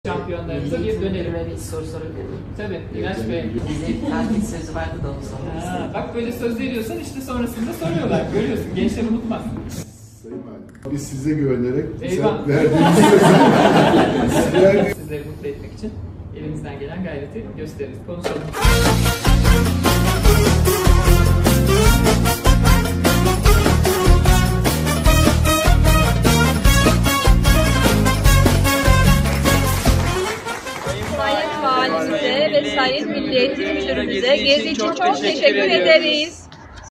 Afyonkarahisarda Vali adına verilen söz yerine geldi
Afyonkarahisar'da LGS’de derece elden öğrencilere Milli Eğitim Müdürü Miraç Sünnetci tarafından Afyonkarahisar Valisi Doç Dr. Kübra Güran Yiğitbaşı adına verilen tatil sözü yerine getirildi.
Onların istekleri yerine geldi ve şampiyonlarımız gönüllerince tatil yaptı.”